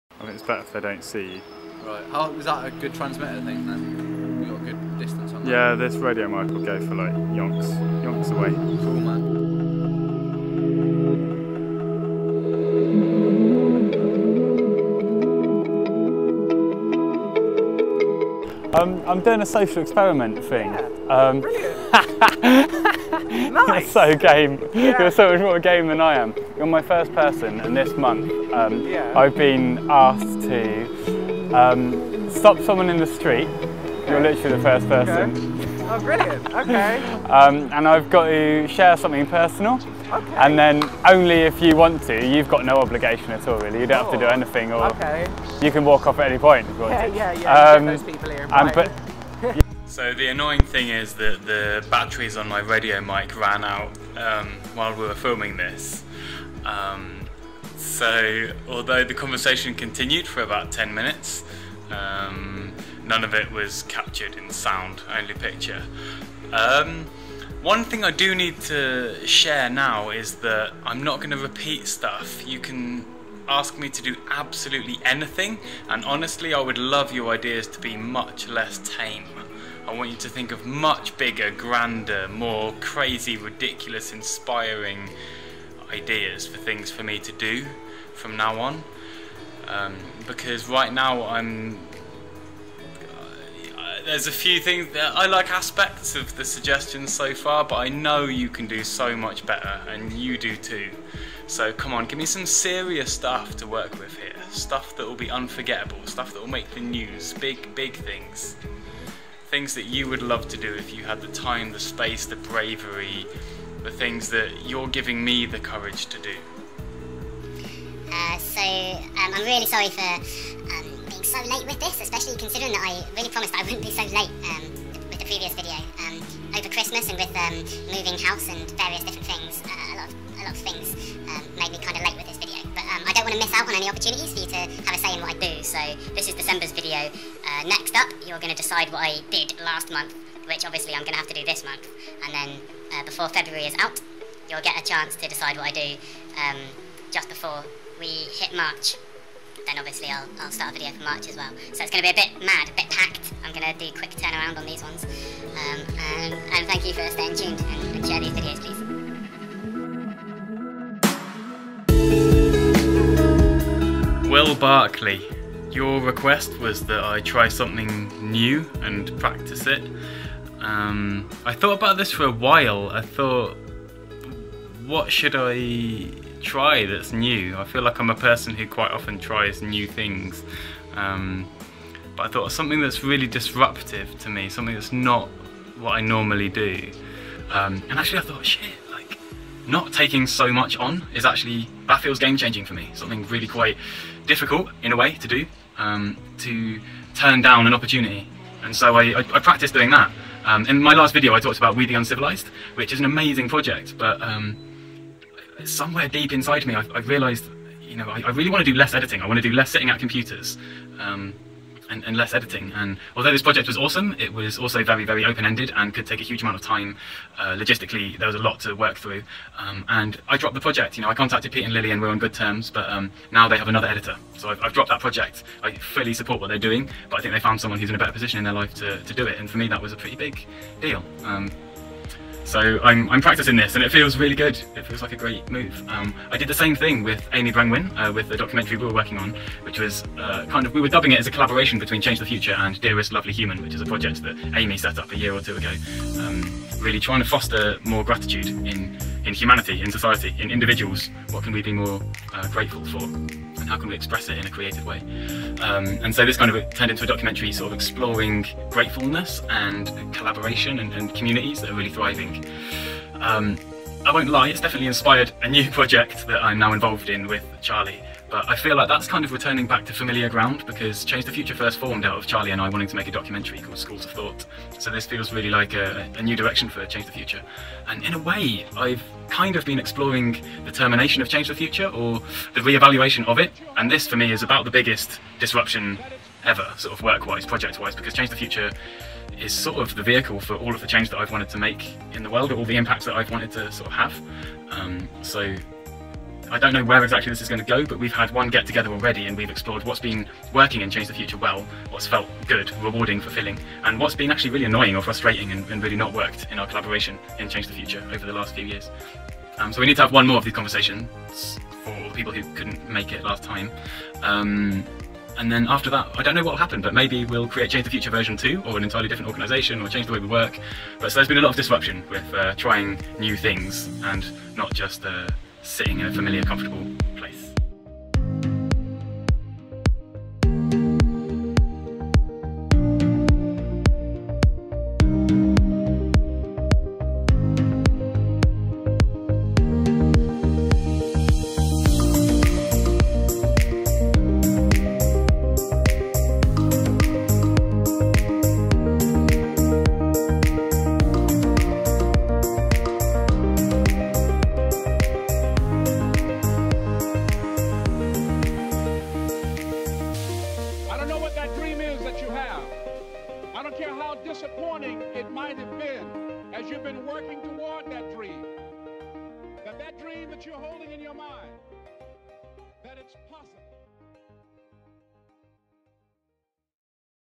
Decide what I do for £1 - December 2015
I invite everyone in the world
to decide what I do next month
for £1.

If you had no fear, what would you do?
I'll be your guinea pig.